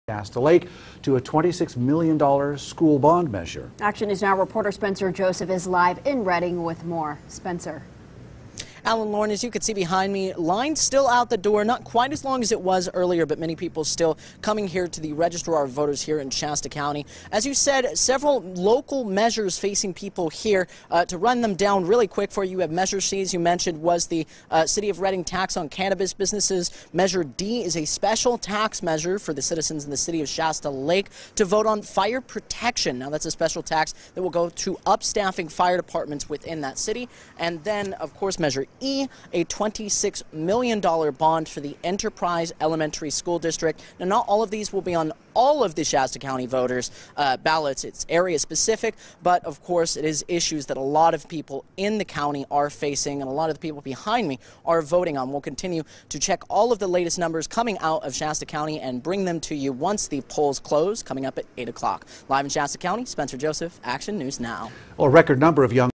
Action News Now Breaks Down Shasta County Local Measures
Spencer Joseph talks about measures C, D and E in Shasta County which are area specific.